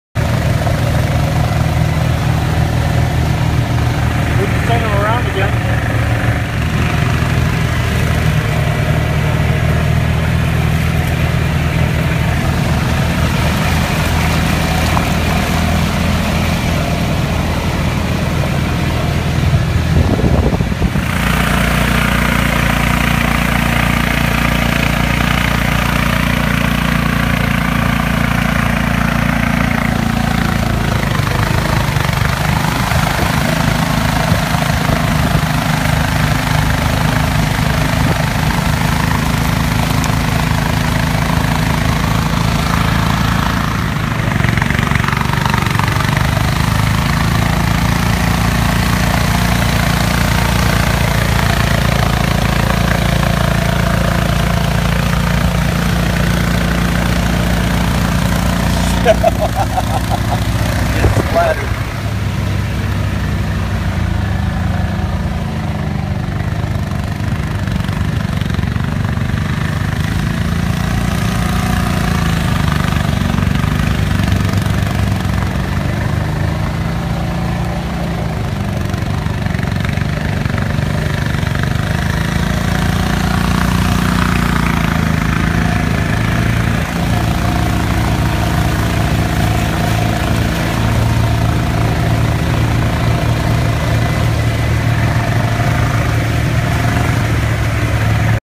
Official BUV Pace Car Demo at the 2015 BUV Competition
This is the official BUV pace car from the Institute of Affordable Transportation. We had a chance to film it in operation in some of the rainiest, muddiest conditions possible. Thanks to the big tractor tires on the rear, it didn't get stuck during the entire 6 hour event. for more information.